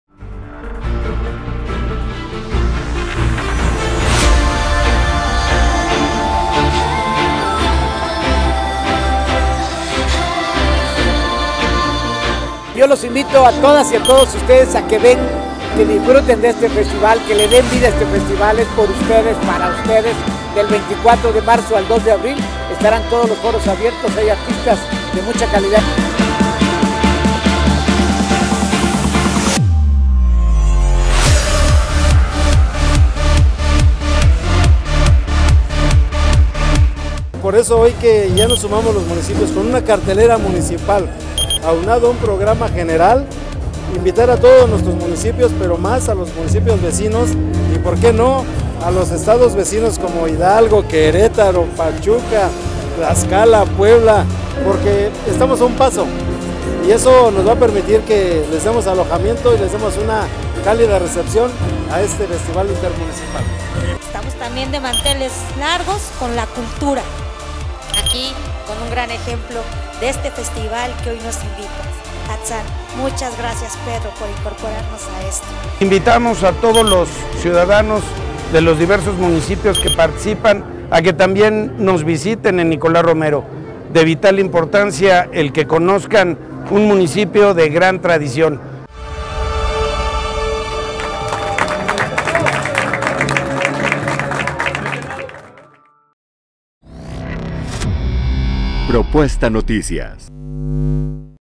POR PRIMERA VEZ ATZÁN SERÁ FESTIVAL INTERMUNICIPAL
*Gracias a la cooperación, respeto y buen entendimiento entre Atizapán, Nicolás Romero, Tlalnepantla y Cuautitlán Izcalli, habrá más de 150 eventos artísticos y culturales en esos municipios.
*En un gesto de generosidad, el alcalde atizapense Pedro Rodríguez, contribuye a que el evento salga de Atizapán, para que también sea disfrutado por vecinos de otras localidades.

ATIZAPÁN, Méx.- El clima de cooperación, respeto, buena comunicación y dejando de lado ideologías y colores partidistas, hizo posible que este municipio, Nicolás Romero, Tlalnepantla y Cuautitlán Izcalli, lanzaran de manera inédita un festival cultural y artístico en modo intermunicipal, por lo que “Atzán”, ahora estará presente en cuatro ciudades mexiquenses, del 24 de marzo al 2 de abril.

Y es que en esta región en la que habitan más de 2 millones de personas, los ediles Pedro Rodríguez, Armando Navarrete, Karla Fiesco y Marco Antonio Rodríguez, han conseguido ir más allá de las diferencias políticas, consiguiendo importantes acuerdos en beneficio de sus gobernados, de lo que son ejemplos la creación de la Policía Intermunicipal, al igual que el carril reversible intermunicipal.

El siguiente paso que han dado dichos presidentes municipales, ha sido extender el arte y la cultura mediante un festival que originalmente surgió en Atizapán, entonces denominado “Luminaria”, hoy Atzán, que presentará más de 150 eventos de alta calidad.

En esta ocasión, el alcalde atizapense Pedro Rodríguez, determinó compartir ese festival que ha alcanzado reconocimiento a nivel nacional e internacional con sus municipios vecinos, en un gesto de generosidad y con el propósito de que dicho evento trascienda, haciendo historia.

Los alcaldes se reunieron en el Teatro Zaragoza para develar la cartelera que anuncia eventos de primer nivel como la presentación del cantante Emmanuel, quien abrirá el festival la noche del 24 de marzo en el escenario instalado a un costado del Palacio Municipal de Atizapán de Zaragoza.

En su intervención, el edil Pedro Rodríguez Villegas, destacó que este primer festival intermunicipal se creó gracias a la suma de voluntades y una visión amplia de gobierno que posibilita la vecindad de los municipios.

El Alcalde atizapense destacó que en Atizapán de Zaragoza participarán artistas como Emmanuel, Fernando de la Mora, Mario Bautista, Matute y se presentará un tributo a la banda sueca Abba, entre otros.

Por su parte, la Alcaldesa de Cuautitlán Izcalli, Karla Leticia Fiesco García, concordó con el Alcalde de Atizapán respecto de la cercanía municipal y los trabajos conjuntos que se realizan.

En tanto, Rodolfo López Olvera, Secretario del Ayuntamiento de Nicolás Romero, a nombre del alcalde Armando Navarrete López, dijo que el Municipio se ha caracterizado por impulsar la cultura y que presentarán, entre otros, a los Grupos La Changa y los Askis, así como un videomapping que compartirán con las demás demarcaciones, en el cual se narra la historia de los 10 pueblos originarios.

Finalmente, Marco Antonio Rodríguez Hurtado ponderó la voluntad por trabajar de manera conjunta en la región y de unirse en el tema cultural y tener una buena vecindad. “Cada uno de nuestros municipios tiene una riqueza cultural e histórica sumamente importante y que muchas veces no la descubrimos estando tan cerca. Hoy nos permite el Presidente Pedro ser parte de un proyecto como este y a veces no nos damos el tiempo de compartir la riqueza cultural, además de los grandes artistas que tenemos”, expresó Rodríguez Hurtado.